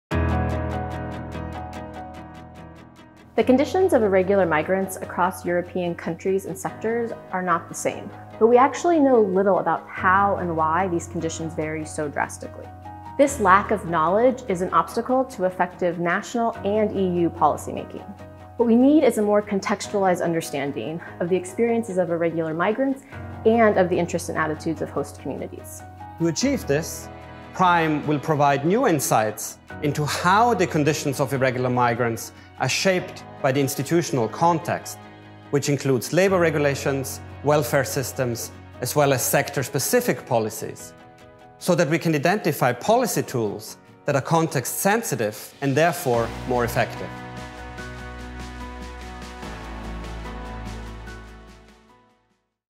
About | Protecting Irregular Migrants in Europe project
Protecting Irregular Migrants in Europe (PRIME) is an EU-funded international research and policy project that analyses the conditions and politics of irregular migrants in Europe. PRIME conducts research on how national institutions shape the conditions of irregular migrants across Europe to develop contextualised and more effective policy responses.